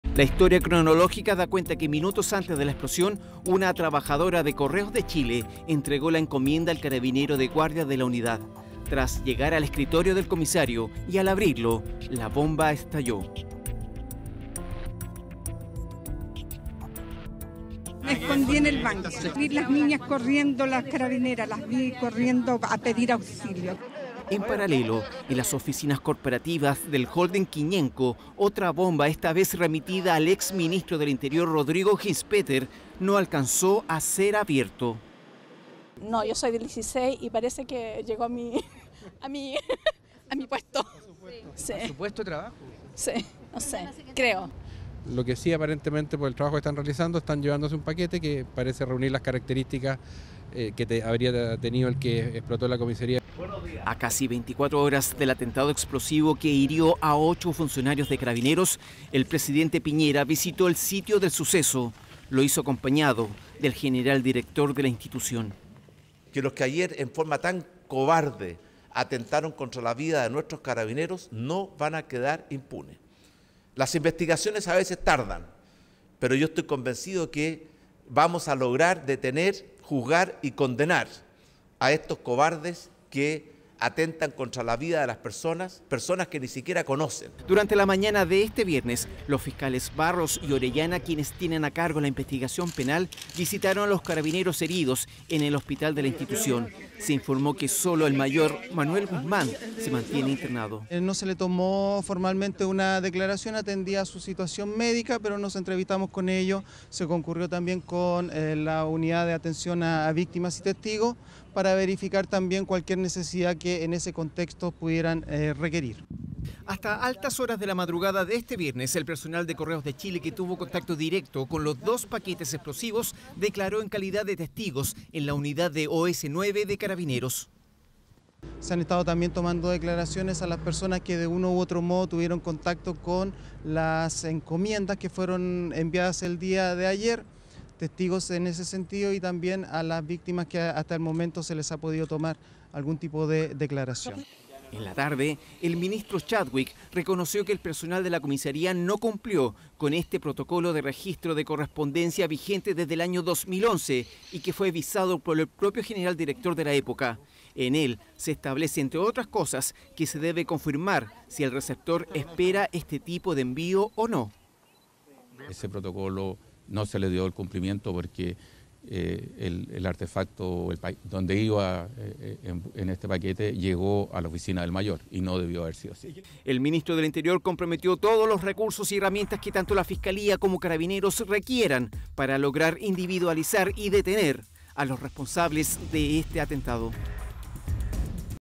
No se cumplió el protocolo establecido por Carabineros para "paquetes sospechosos"
El ministro del Interior Andrés Chadwick admitió que los funcionarios policiales de la 54° Comisaría no cumplieron con el protocolo de recepción de correspondencia vigente desde el 2011, el que establece medidas para paquetes sospechosos. Durante la mañana el Presidente Piñera visitó la unidad donde estalló la bomba, quedando en evidencia el poder destructivo de la explosión.